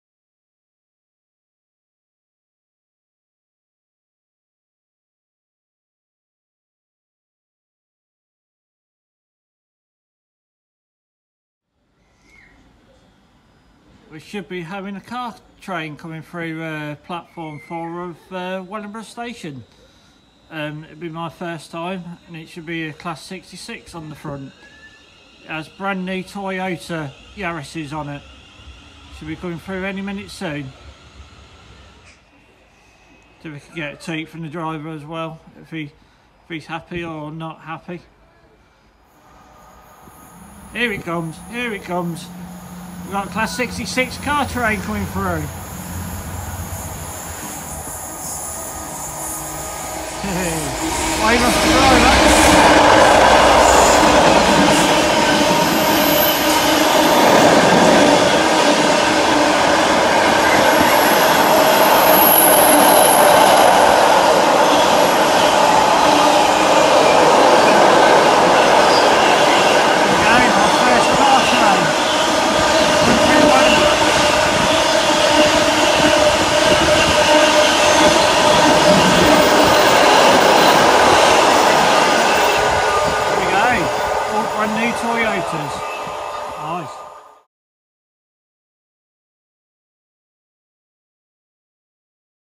My first Toyota car train class 66040 DB CARGO UK 6X11 TOTON TO DOLLANDS MOOR 13/4/22
please enjoy this video off my first Toyota car train going through Wellingborough Station class 66040 DB CARGO UK 6X11 TOTON TO DOLLANDS MOOR please help me get this special video up to 1000k please